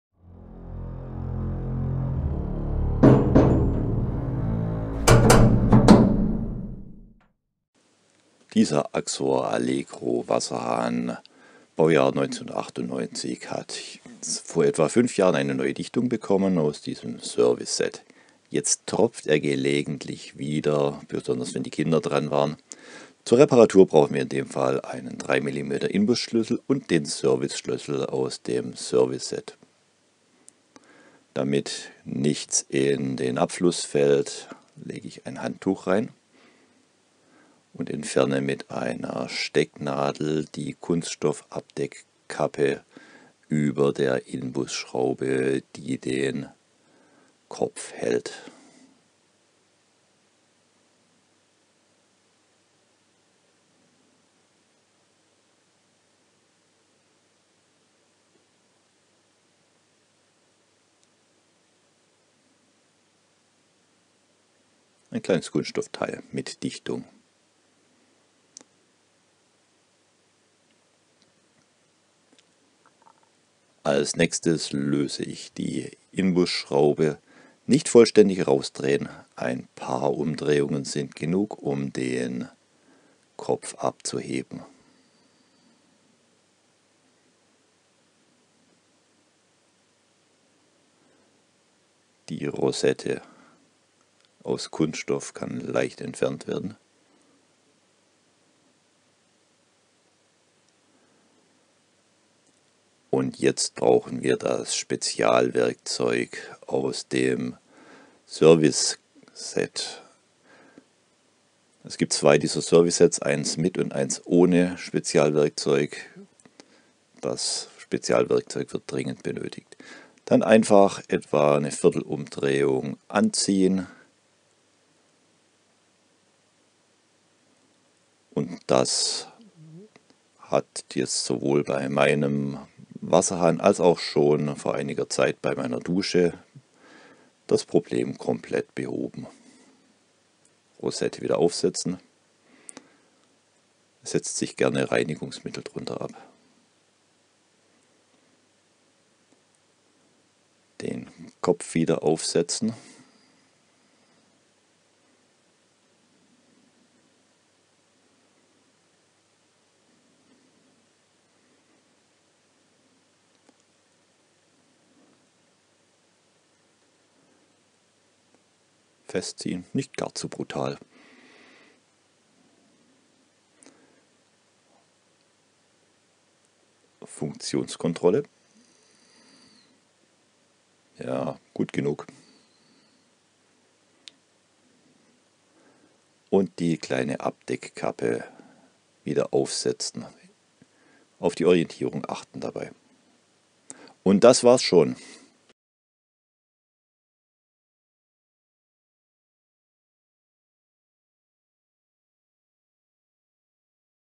Hansgrohe Axor Allegroh Wasserhahn tropft. Einfache Reparatur. (German only)
Einfache Reparatur eines Hansgrohe Axor Allegroh Einhebel Waschtischmischers mit dem Serviceschlüssel aus dem Service Set 13952000 (ca 20€)
Die Dichtungen unterhalb der Steuerkugel hatte ich vor etwa 5 Jahren getauscht. Mit dem im Service Set enthaltenen Schlüssel konnte ich auch ohne Tausch der Dichtungen den Unterputz Einhebel-Brausenmischer aus der selben Serie wieder dicht bekommen.
Hinweis: Ich stehe in keiner Verbindung von Fa. Hansgrohe und biete keine Dienstleistung an. Ich habe nur meinen Wasserhahn repariert.